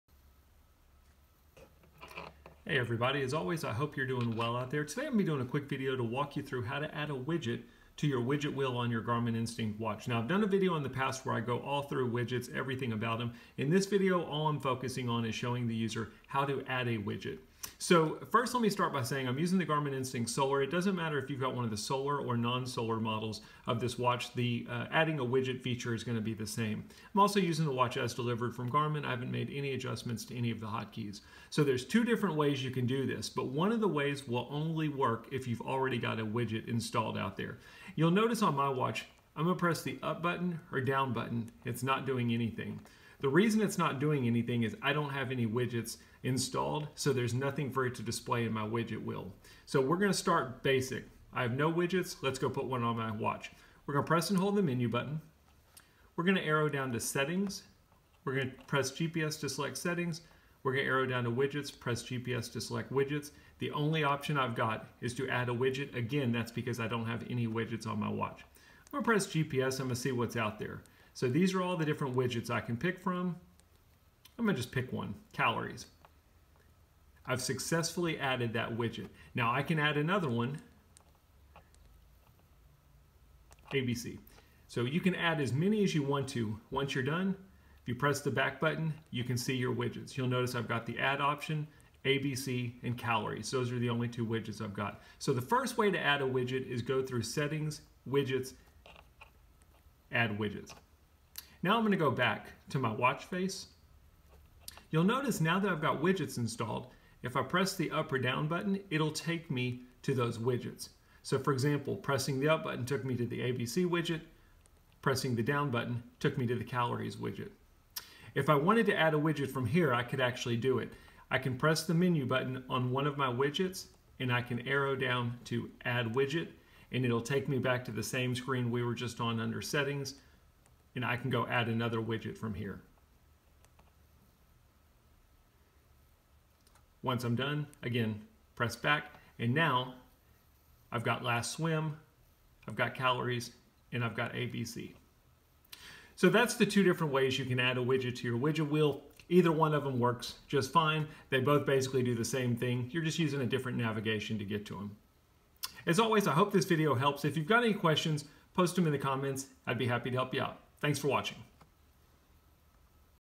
Garmin Instinct - Add a Widget
How to add a widget on the Garmin Instinct.